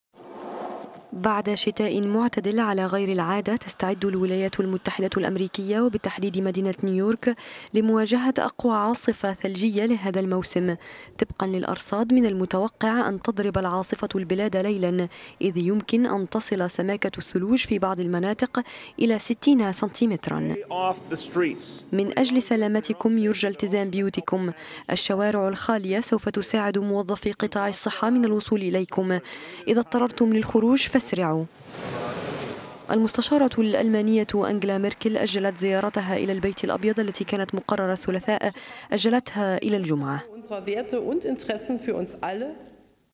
ميركل تؤجل زيارتها للبيت الأبيض بسبب العاصفة الثلجية
بعد شتاء معتدل على غير عادة، تستعد الولايات المتحدة و بالتحديد مدينة نيويورك لمواجهة أقوى عاصفة ثلجية لهذا الموسم. طبقا للأرصاد، من المتوقع أن تضرب العاصفة البلاد ليلا إذ يمكن أن تصل سماكة الثلوج في بعض المناطق الى ستين سنتيمترا.

رئيس بلدية نيويورك، بيل دي بلاسيو قال:" من أجل سلامتكم يرجى التزام بيوتكم، الشوارع الخالية سوف تساعد موظفي قطاع الصحة من الوصول إليكم، إذا اضطررتم للخروج فاسرعوا".

المستشارة الألمانية انجيلا ميركل أجلت زيارتها إلى البيت الأبيض التي كانت مقررة الثلاثاء.